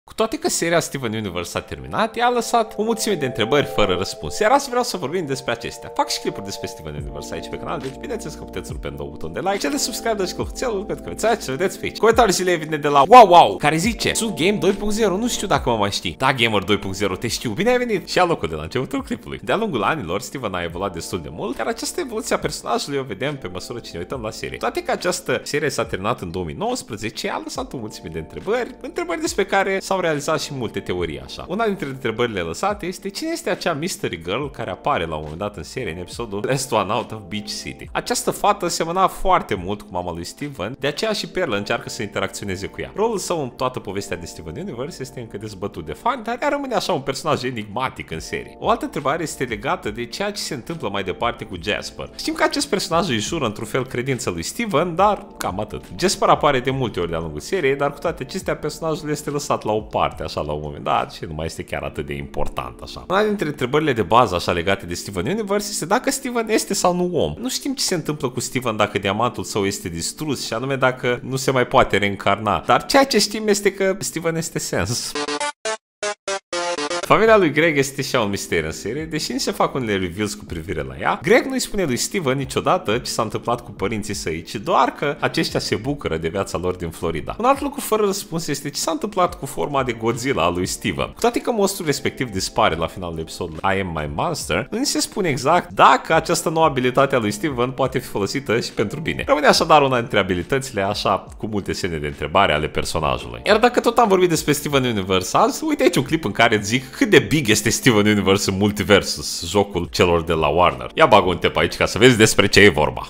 5 INTREBARI FARA RASPUNS DIN STEVEN UNIVERSE!
Azi vorbim despre Steven Universe si cateva intrebari fara raspuns!
⛏ SERVERUL DE MINECRAFT AL CANALULUI: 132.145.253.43